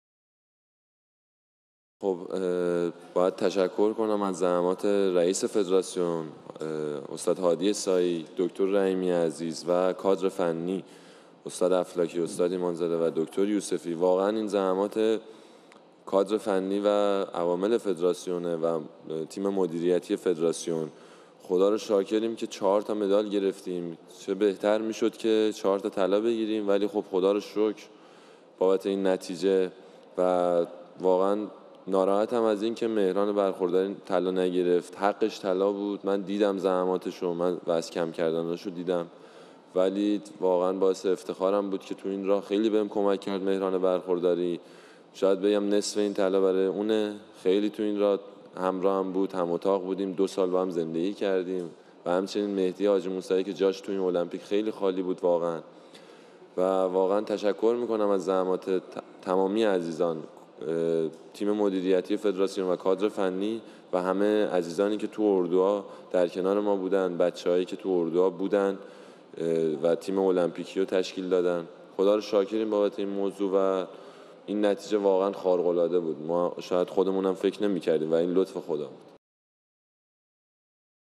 "We all supported one another." - Salimi after making it 4 out of 4 for Iran.
Reactions after Iran's Arian Salimi defeated Great Britain's Caden Cunningham 2-1 in a very tight men's +80kg taekwondo final, on Saturday.

Bronzes went to Rafael Alba of Cuba and Cheick Sallah Cisse of Cote d'Ivoire.